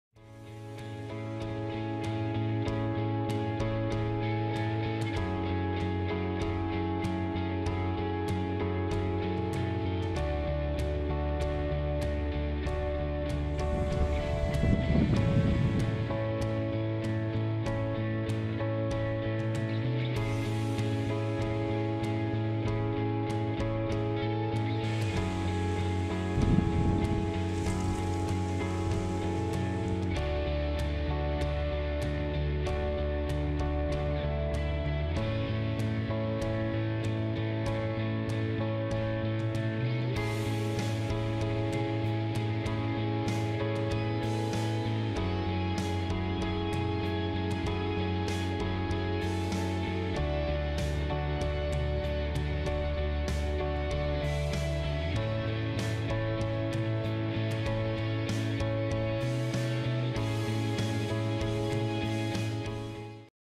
Steinfeldsee
Atemberaubende Luftaufnahmen der Steinfeldseen im Harzgebirge